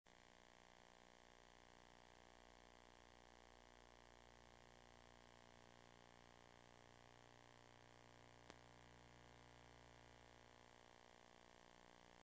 MCA Home Video (1960) Company Logo (VHS Capture)
Company Logo for "MCA Home Video (1960)" captured from the "Psycho (1960)" VHS tape. This tape is labeled 55001 and released in (1995).